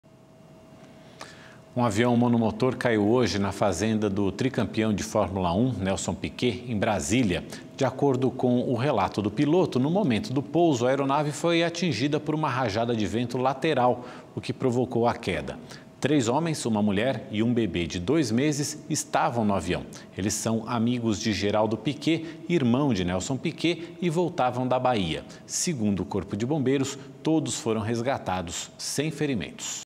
Avião cai na fazenda do ex-piloto Nelson Piquet em Brasília
Um avião monomotor caiu, na fazenda do tricampeão de fórmula um, Nelson PIquet, em Brasília. De acordo com o relato do piloto, no momento do pouso, a aeronave foi atingida por uma rajada de vento lateral, o que provocou a queda.